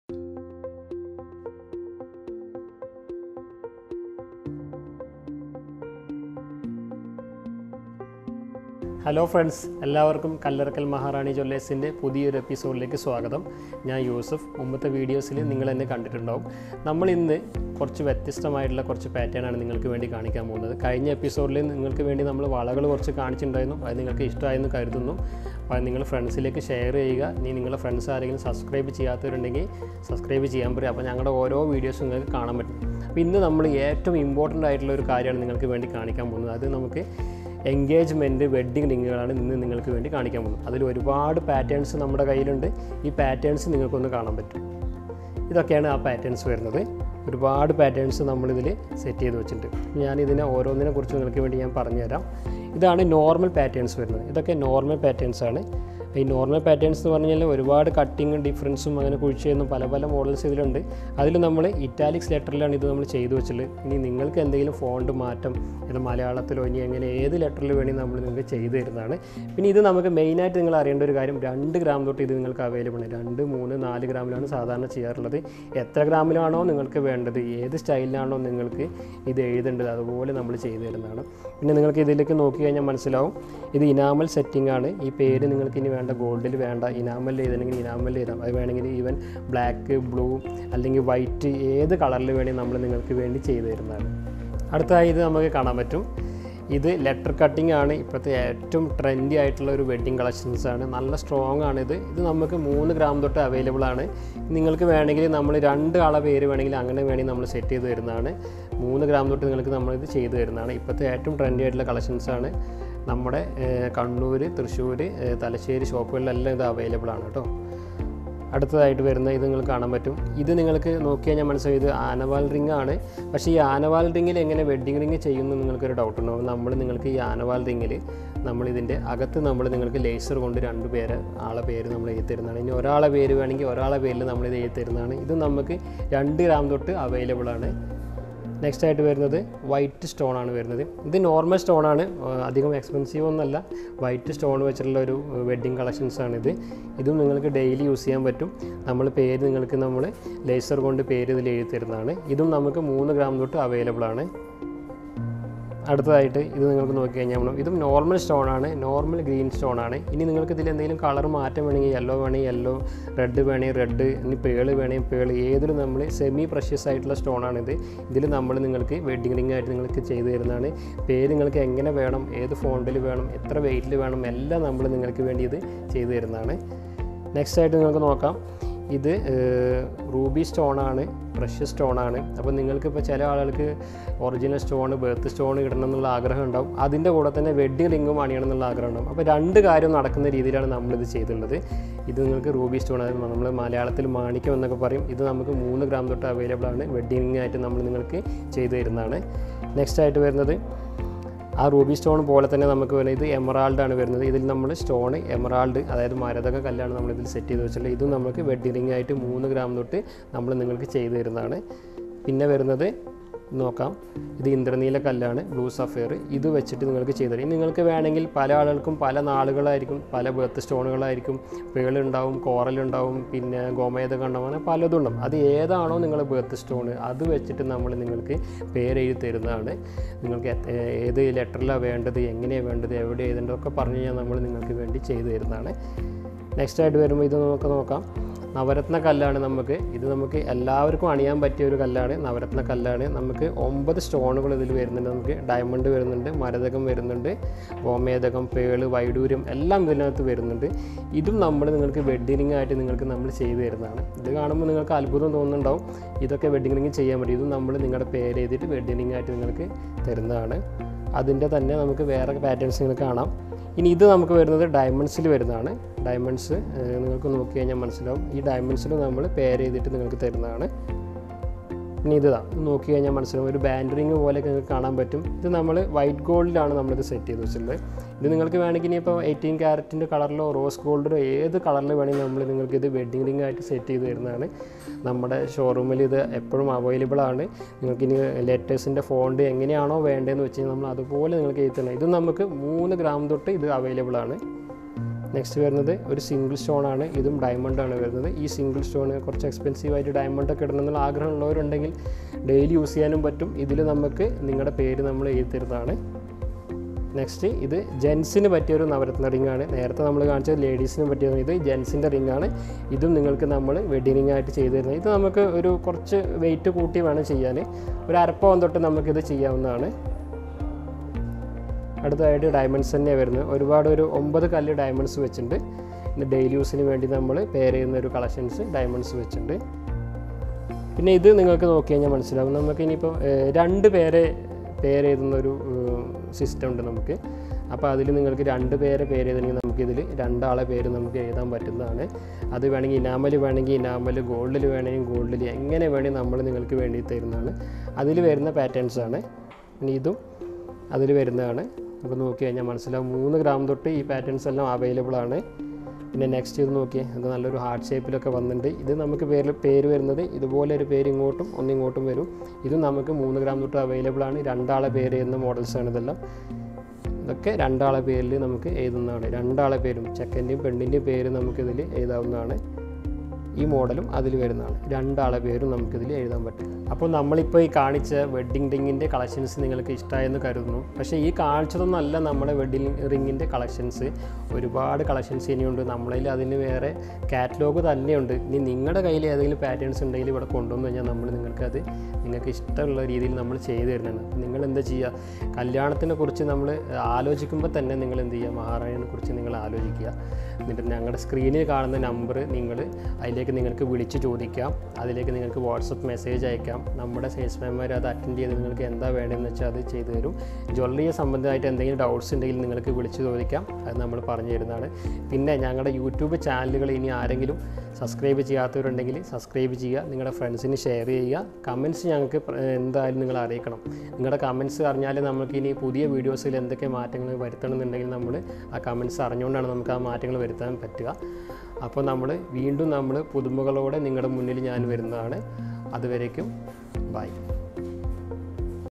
Wedding Ring Collection | കല്യാണ മോതിരത്തിൻ്റെ ഒരു അതി വിപുലമായ കളക്ഷൻ
Wedding rings for the special occasion with different customization options as per your requirements are available at our showrooms | കല്യാണത്തിന് ആവശ്യമായ വിവിധ മോതിരങ്ങൾ നിങ്ങൾക്കു ഇഷ്ടപ്പെടുന്ന രീതിയിൽ മാറ്റങ്ങൾ വരുത്തി പണിതെടുക്കാവുന്നതാണ് നമ്മുടെ ഷോറൂമുകളിൽ